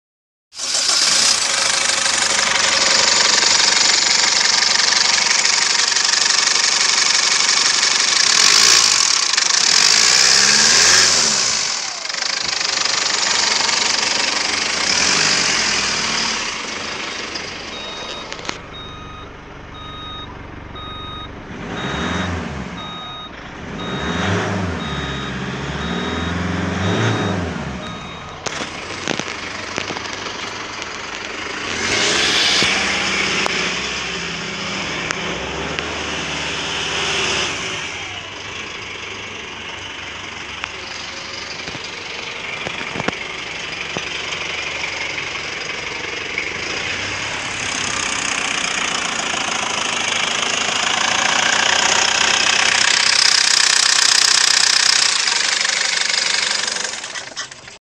2016 FOTON TUNLAND 2.8DT ENGINE TEST NMW07249
SPARE CAR PARTS AVAILABLE FOR FOTON TUNLAND. ENGINE TESTED GOOD.